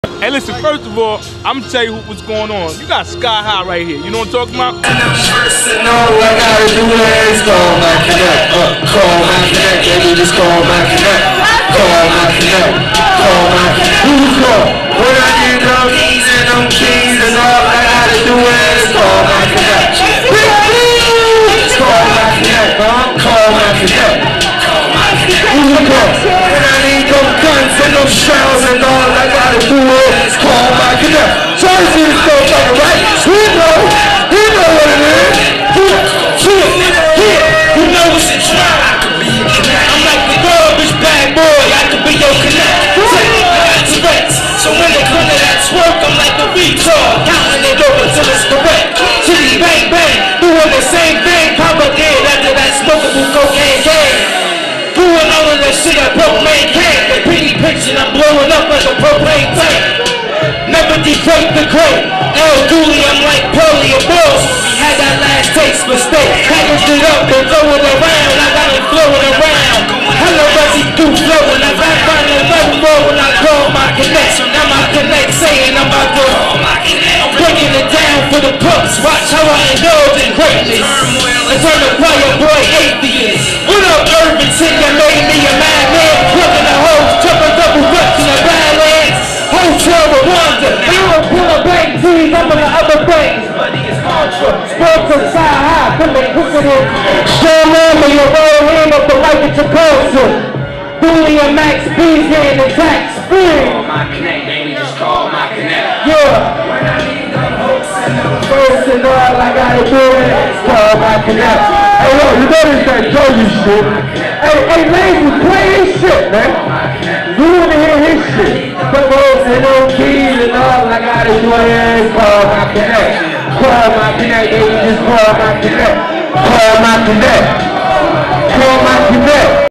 Skyy High
Jerzey in The Building! Skyy High Performs wm Max B (formerly of dipset). Live from Suga Rays - NWK (Brick City)